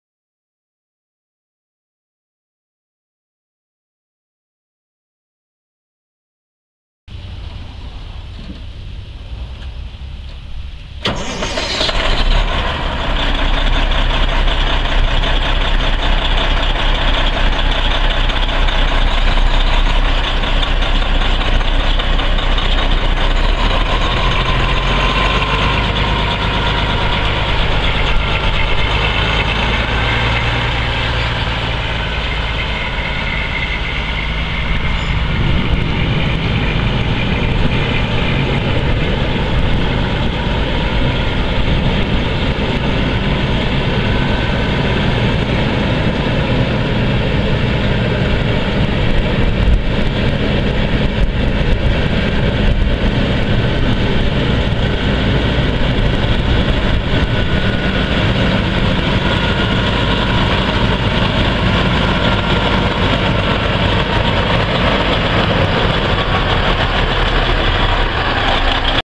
1998 International 9200 Eagle semi truck for sale | sold at auction August 20, 2013
1998 International 9200 Eagle semi truck
337,770 miles on odometer
Detroit Diesel Series 60 12.7L L6 diesel engine
Rockwell 10 speed manual transmission
Twin screw
AC and heat
Tilt
Cruise
AM/FM/cassette
Passenger side power window
Spotlight
ProSleeper 40" sleeper
Storage
Visor
Chrome bumper
Block heater
Poly quarter fenders
Dump valves
Air suspension
Air brakes
Brake controller
Jake brakes
234" wheelbase
Aluminum fornt wheels
295/75R22.5 tires
Front bumper damage
Paint damage
Fiberglass damage
Steering wheel damage

It was located in Ulysses, KS, and we sold it to the highest bidder for $8,470.